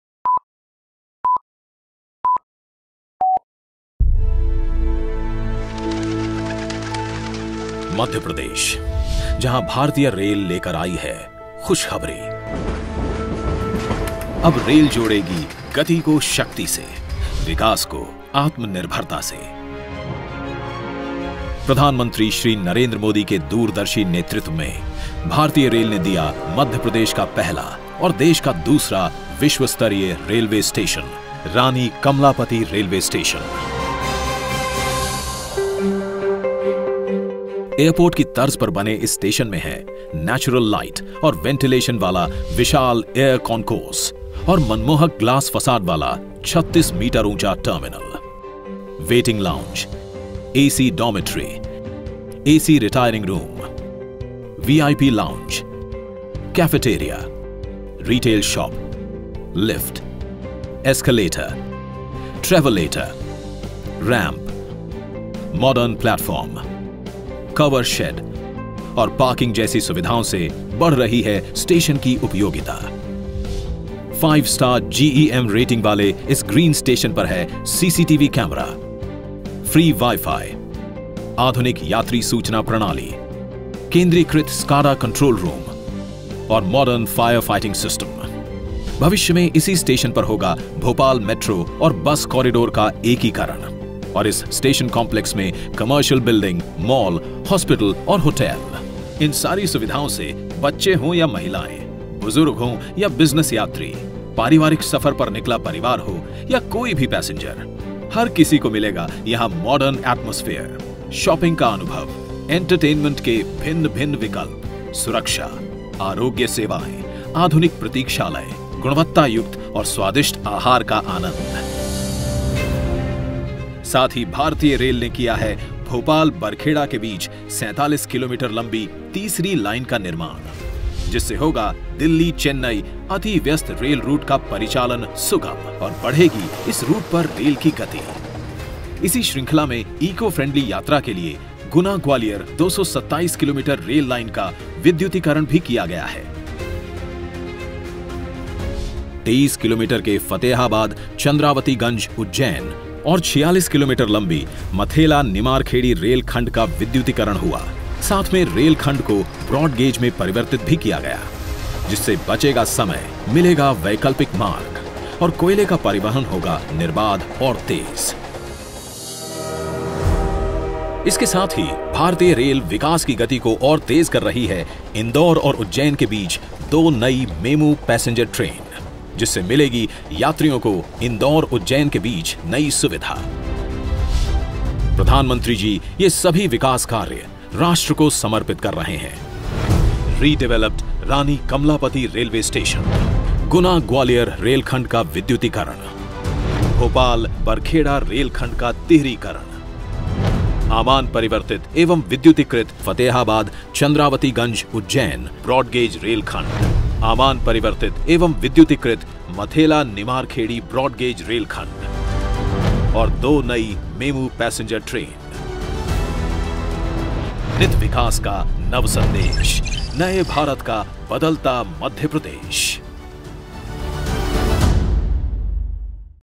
Boosting rail connectivity, Enhancing travel! Redeveloped Rani Kamalapati Station & other projects
During his visit to Madhya Pradesh on 15th November, Prime Minister Narendra Modi inaugurated the redeveloped Rani Kamalapati Railway Station.

The redeveloped Rani Kamalapati Railway station, named after brave and fearless Queen Kamalapati of the Gond kingdom, is the first world class railway station in Madhya Pradesh. Redeveloped in Public Private Partnership (PPP) mode, the station has been designed as a green building with modern world class amenities that also takes into account ease of mobility for divyangjans. The station is also developed as a hub for integrated multi-modal transport.

During the event, Prime Minister dedicated to the nation multiple initiatives of the Railways in Madhya Pradesh including Gauge Converted and Electrified Ujjain-Fatehabad Chandrawatiganj Broad Gauge section, Third line in Bhopal-Barkhera section, Gauge Converted and Electrified Mathela-Nimar Kheri Broad Gauge section and Electrified Guna-Gwalior section. Prime Minister also flagged off two new MEMU trains between Ujjain-Indore and Indore-Ujjain.